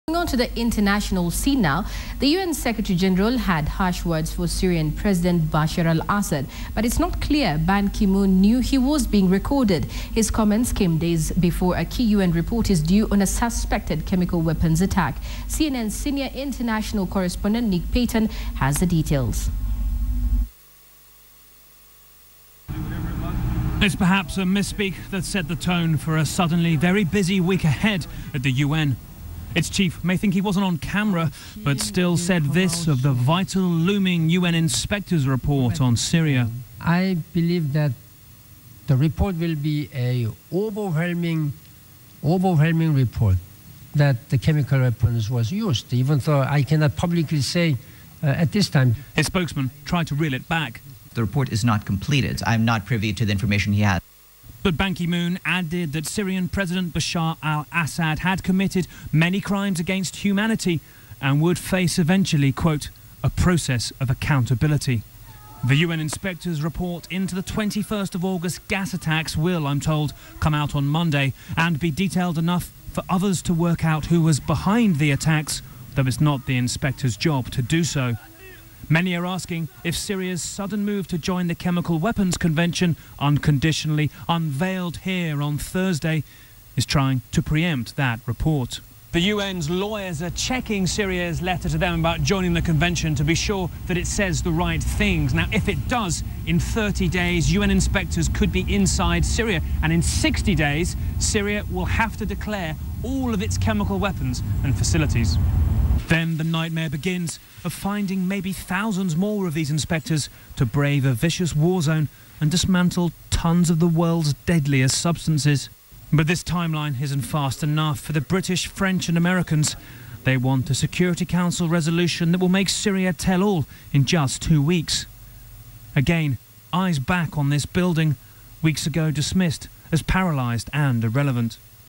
UN Chief Ban Ki-Moon had harsh words for Assad
The U-N secretary general had harsh words for Syrian President Bashar Al-Assad. But it's not clear Ban Ki-Moon knew he was being recorded. His comments came days before a key U-N report is due on a suspected chemical weapons attack